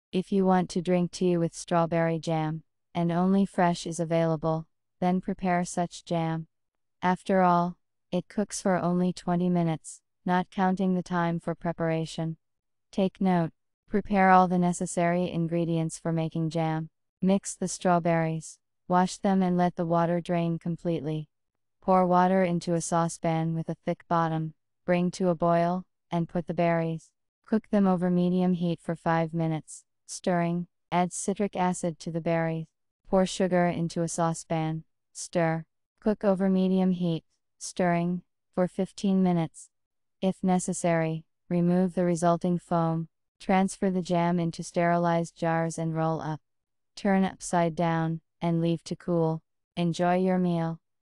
Strawberry jam in 20 minutes
If you want to drink tea with strawberry jam, and only fresh is available, then prepare such jam. After all, it cooks for only 20 minutes, not counting the time for preparation. Take note!

Ingredients:
Water2 Tablespoons,Strawberries 1 Kilogram,
Sugar 1 Kilogram,Citric acid1/3 teaspoons,